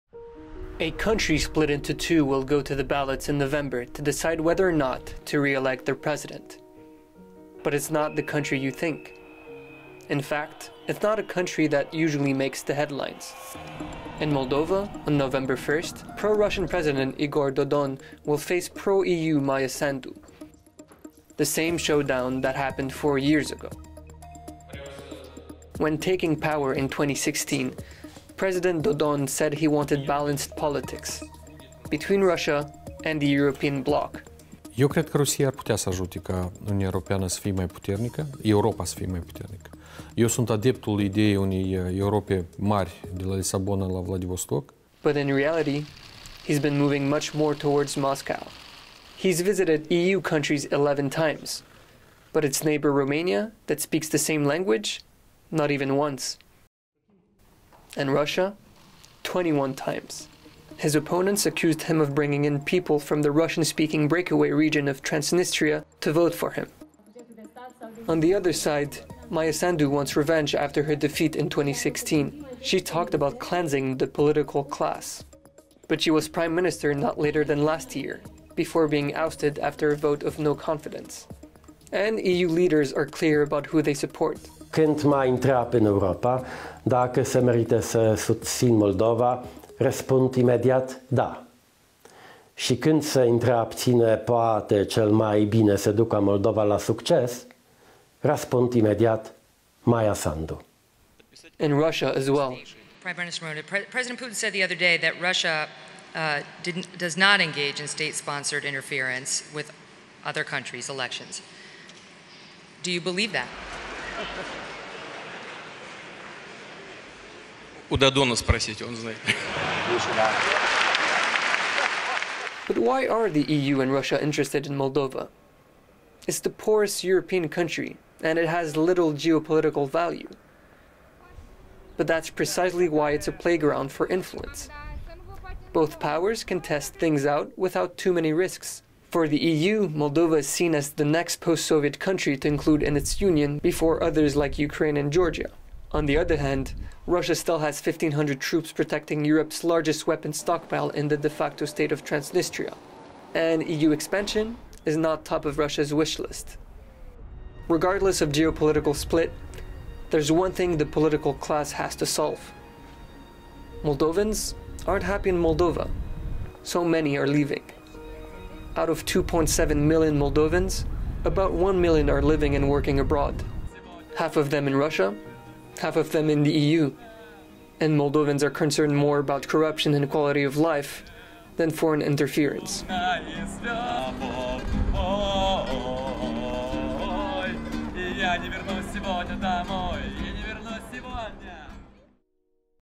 Moldova, a country split between Brussels and Moscow
Moldova's presidential election is set to be a remake of the previous one in 2016. Why are the EU and Russia invested in the race?